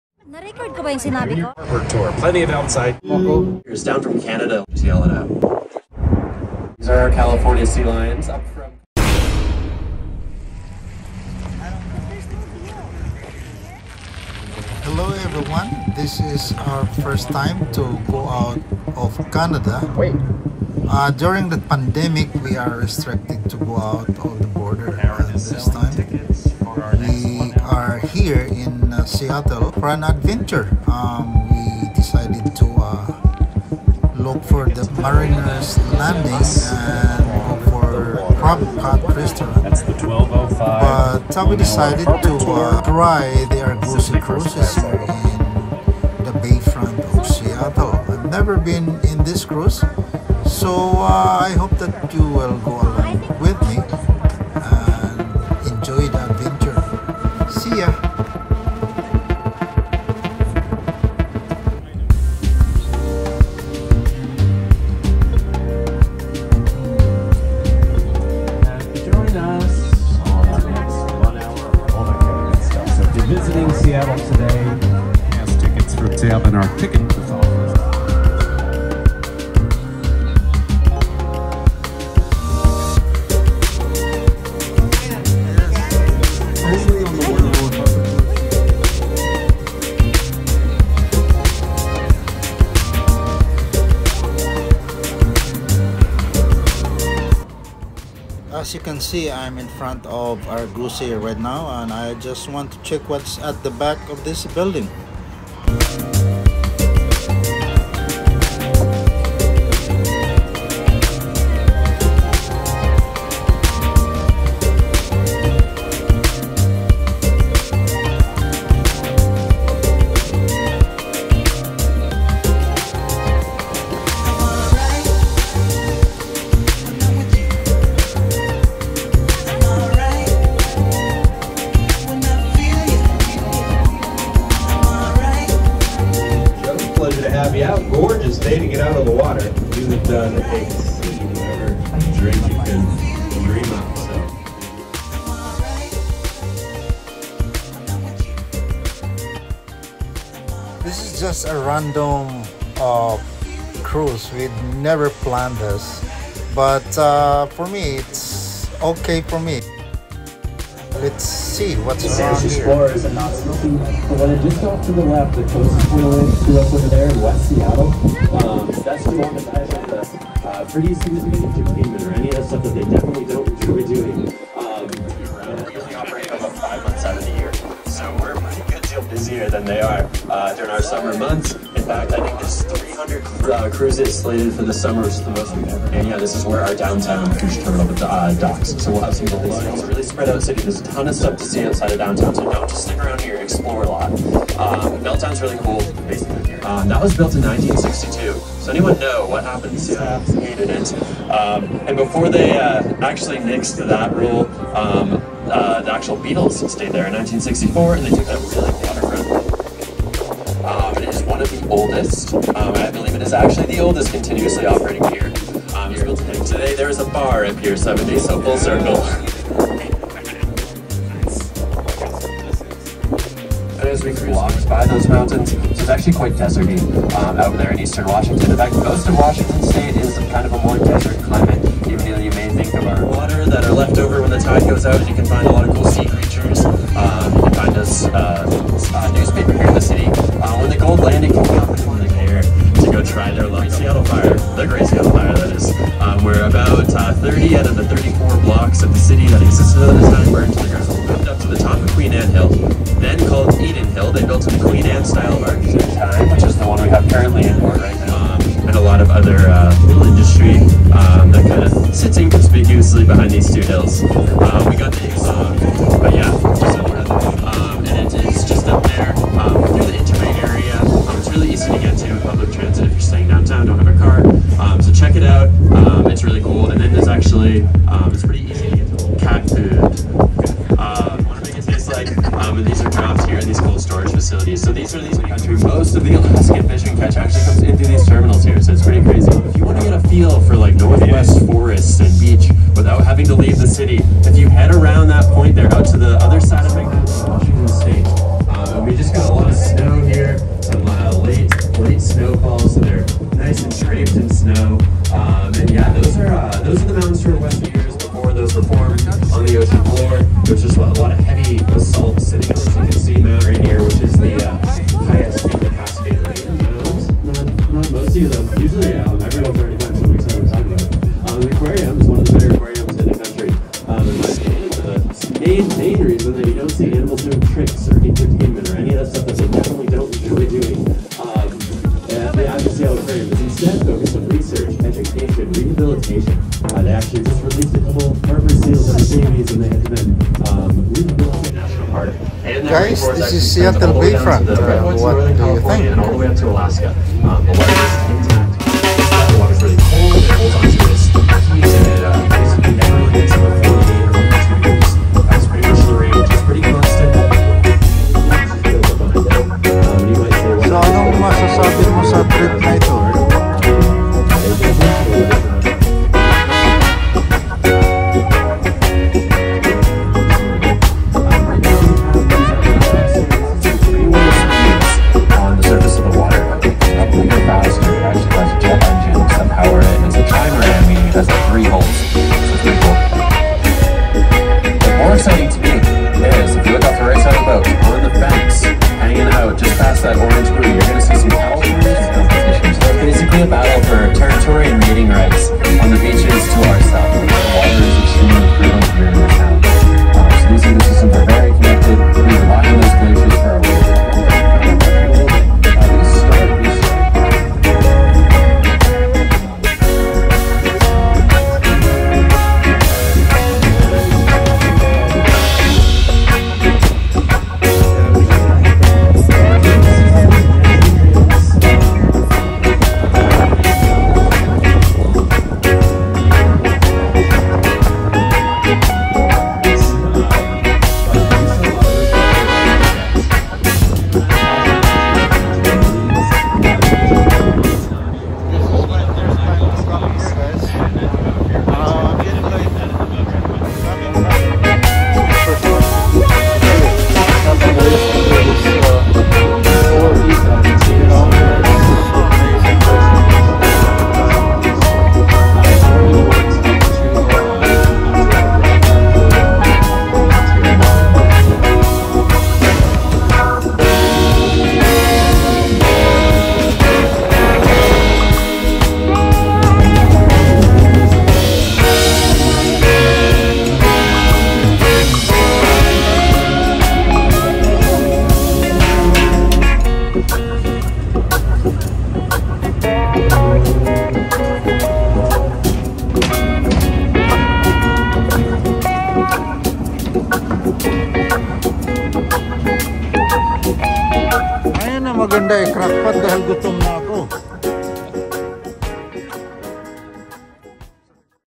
ARGOSY HARBOUR CRUISE ,Seattle Washington 2022
This is a narrated sightseeing harbour tour  and fun on summer time if you visit Seattle bayfront.It will give you informative outlook on the history of Seattle.Great perspective of this historic bayfront and around lake Union. Awesome experience.Check it out folks!

address:
ALASKAN WAY , WAY PIER55 ,SUITE 201,SEATTLE ,WA 98101,USA